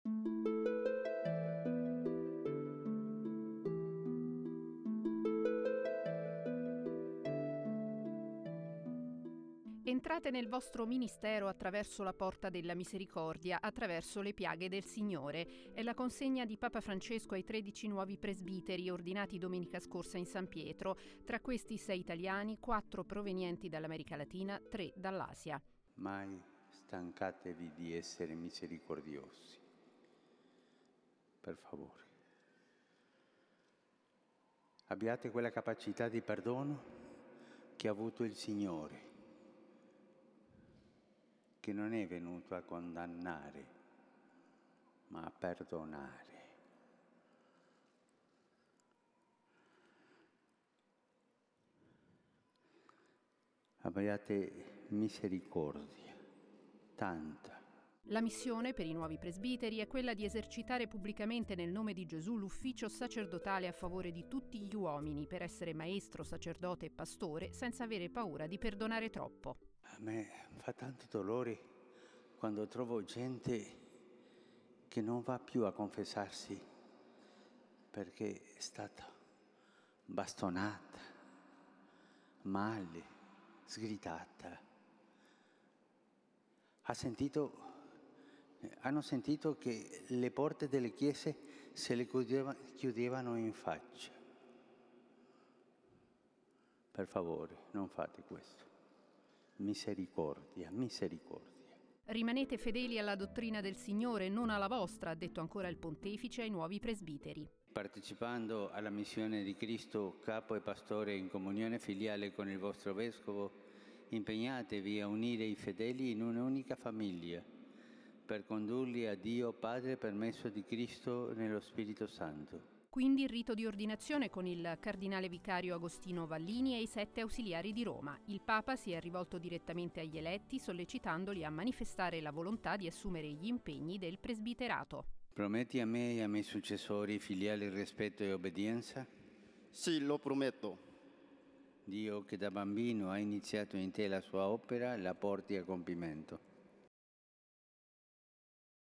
Vatican Magazine " La Porta della Misericordia " 16-05-2014
Papa Francesco ha ordinato 13 nuovi sacerdoti per la diocesi di Roma, invitandoli ad avere "quella capacità di perdono che ha avuto il Signore, che non è venuto a condannare ma a perdonare". Non bastonate o sgridate, ha aggiunto,  chi viene a confessarsi.